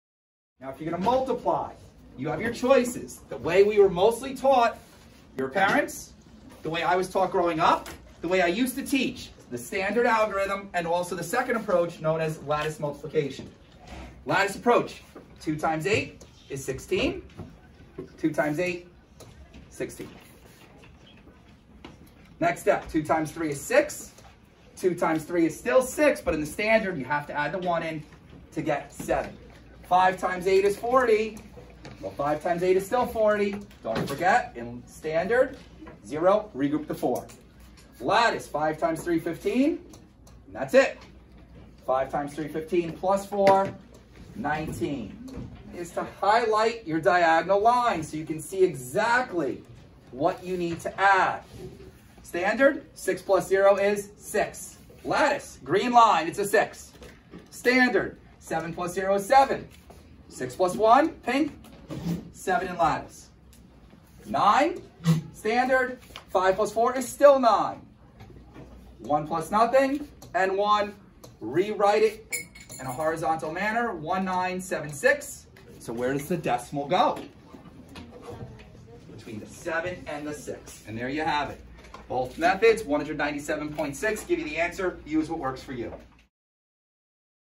Mr Pikarsky Teaching Lattice Multiplication
Hempstead Heroes in Mr. Pikarsky's 5th Grade classroom learned how to multiply using the lattice multiplication method.